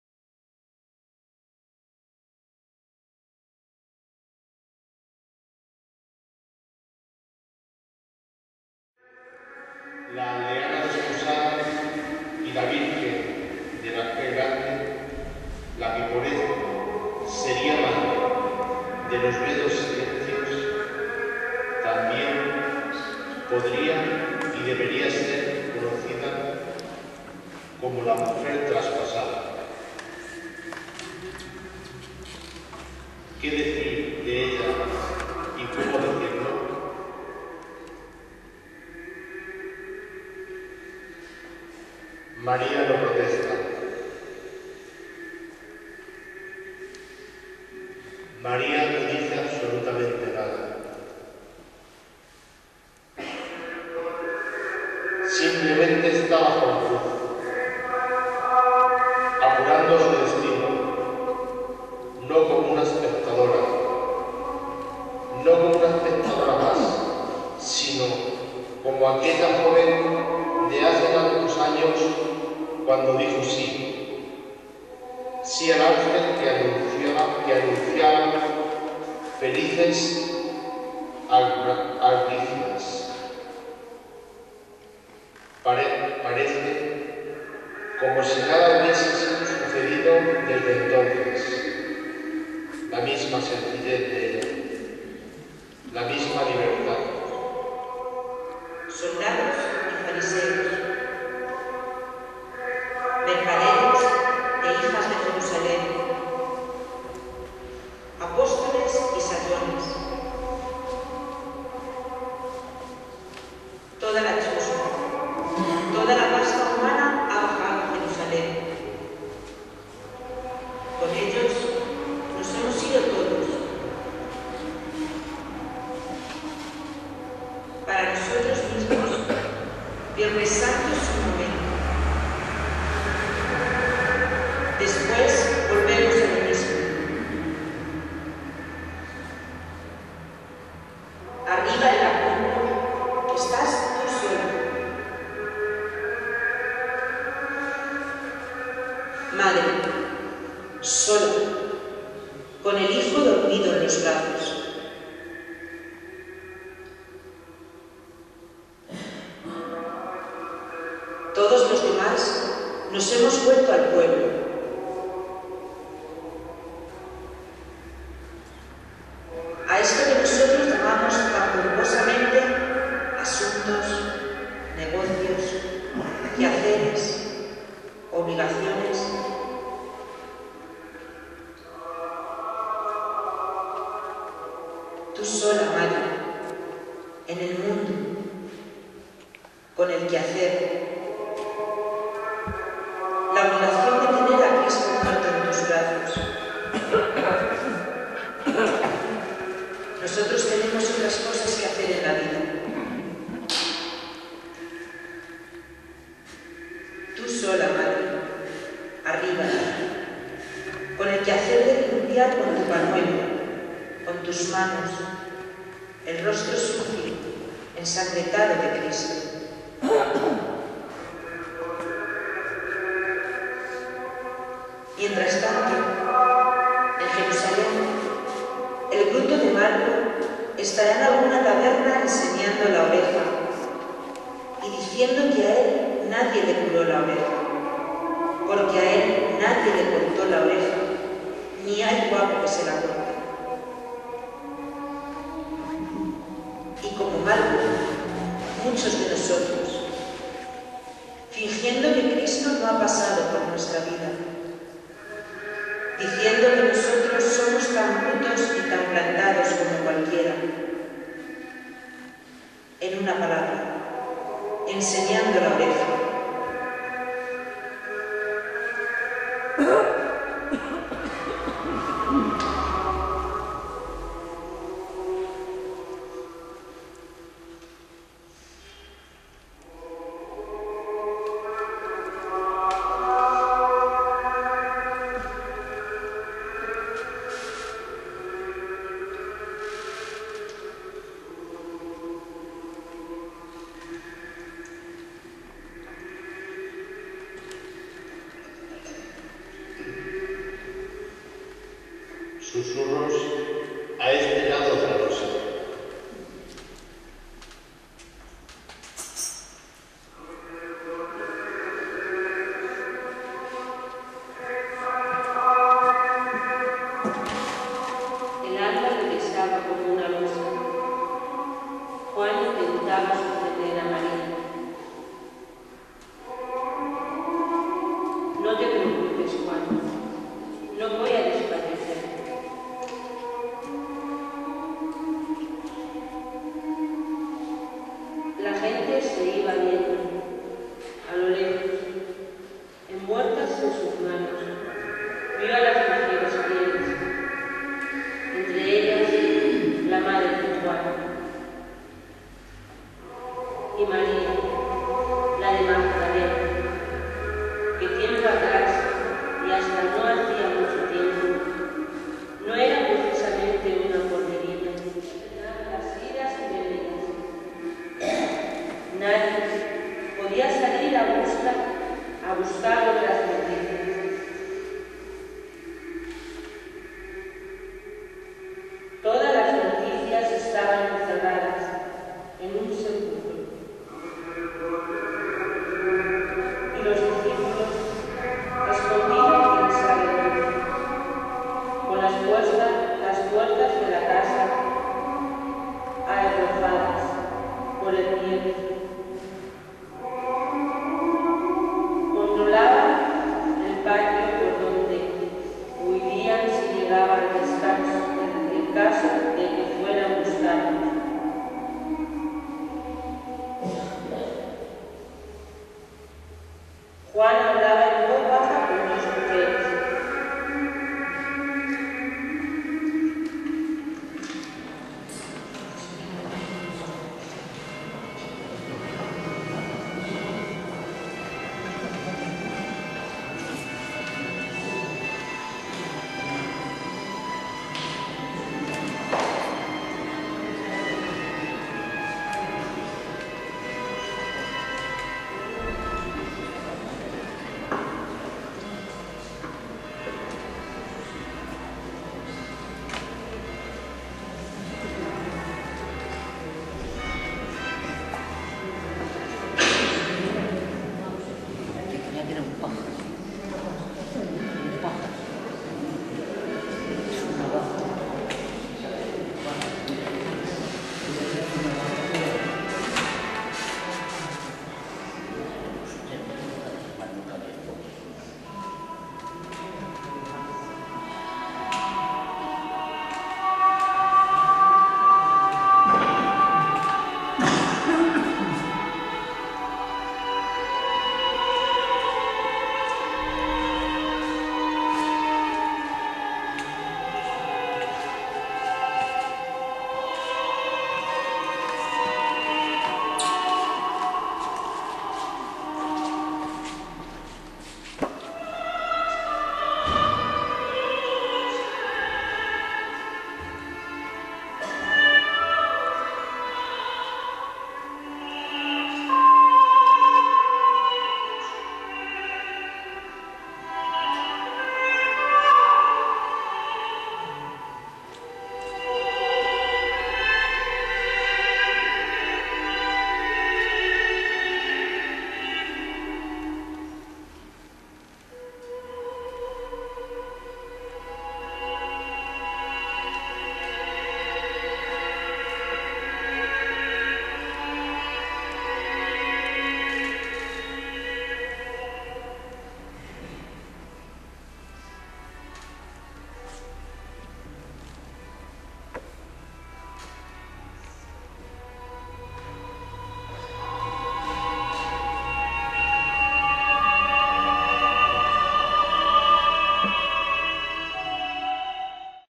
HERENCIA, ORACIÓN MARIANA SABADO SANTO
ORACIÓN MARIANA PREPARADA POR LA ORDEN SEGLAR CARMELITA DE HERENCIA